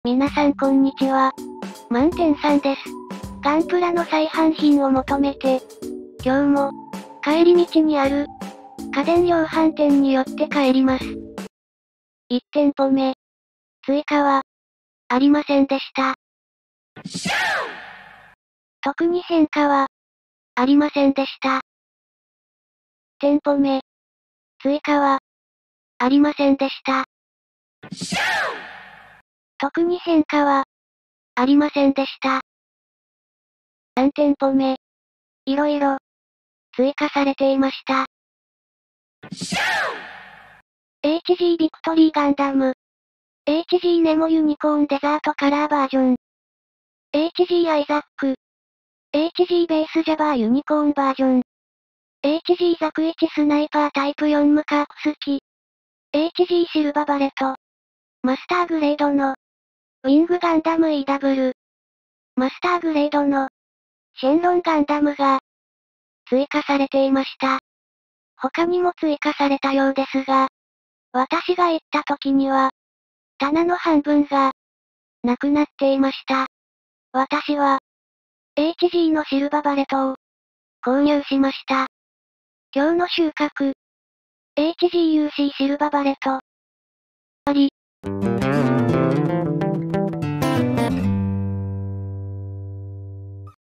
【7/22】ガンプラ再販品を求めて帰宅途中にある家電量販店に毎日寄って帰る。
ガンプラ日記